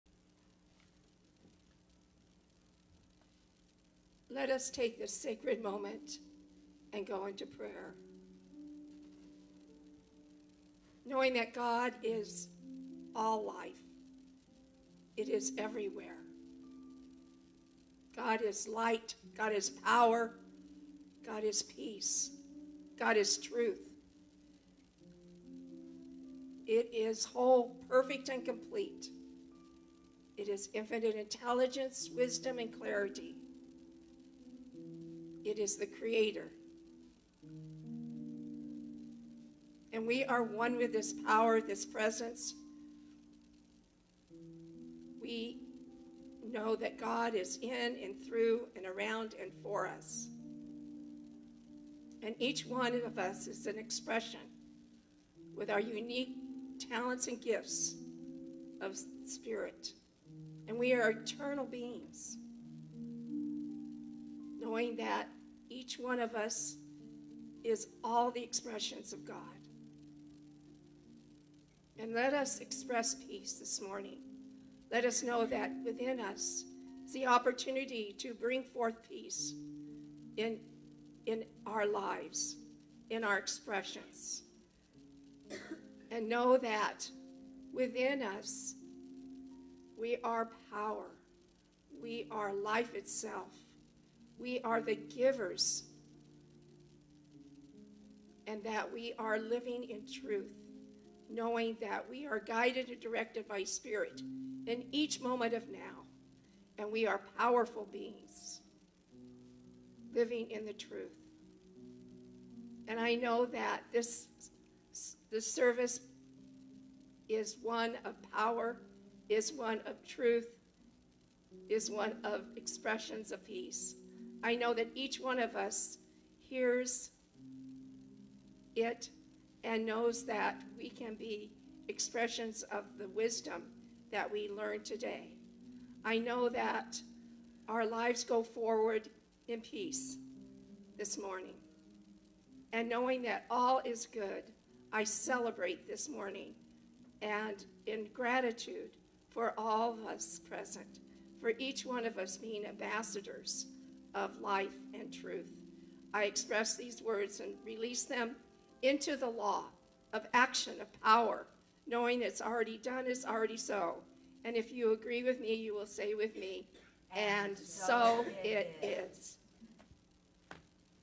Toni Sparks, RScP, gives the invocation SUnday September 22, 2024
If Toni's prayer moved you, please consider donating to our Center using the following link: